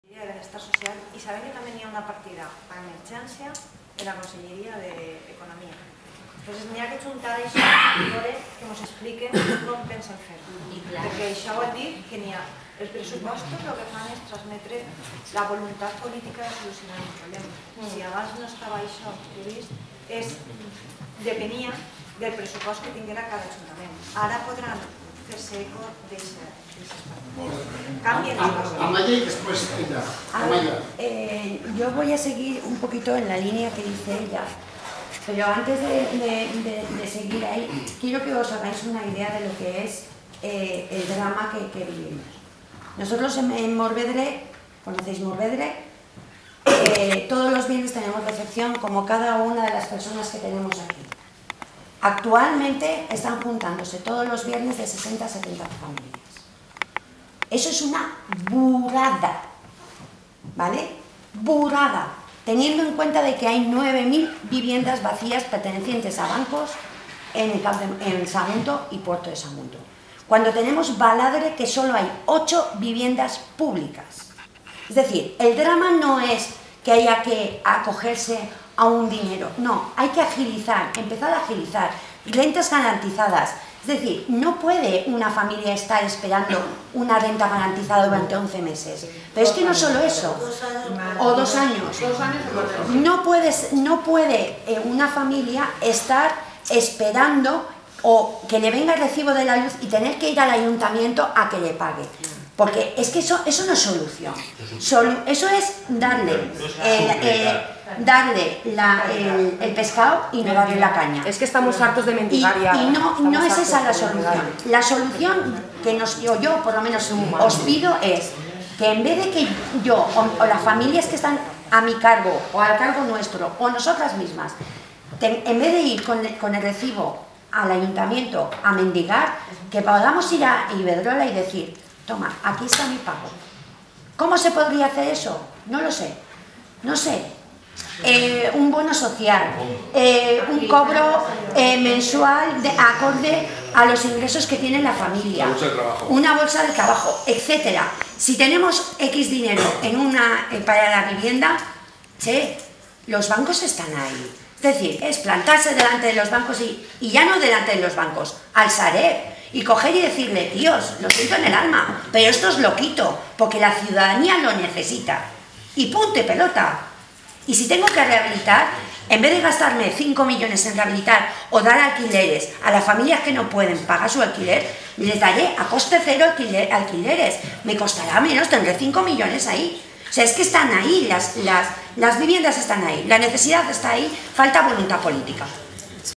PAH en las Cortes parte 3
La PAH en diálogo con las autoridades del gobierno y las cortes valencianas para buscar soluciones a la situación de emergencia social de las familias desahuciadas